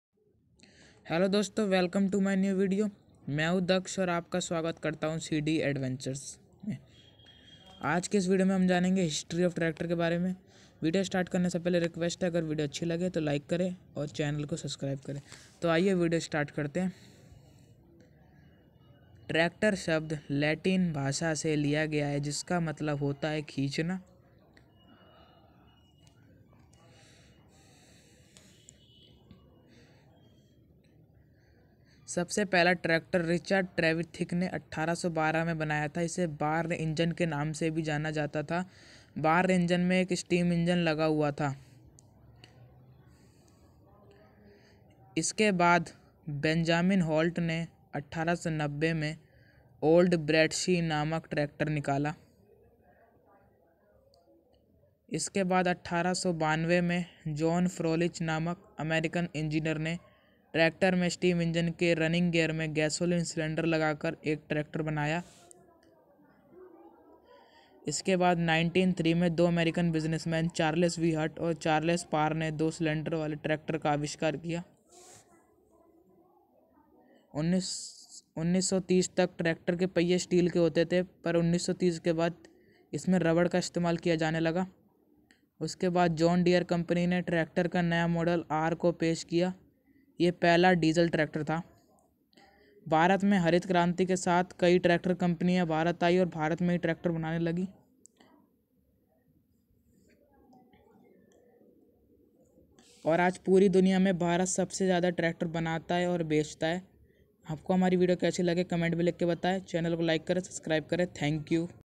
History of Tractor by CD ADVENTURES
hello friends aaj ki is video mai aapko tractor ki history pata chalegi in only 2 minutes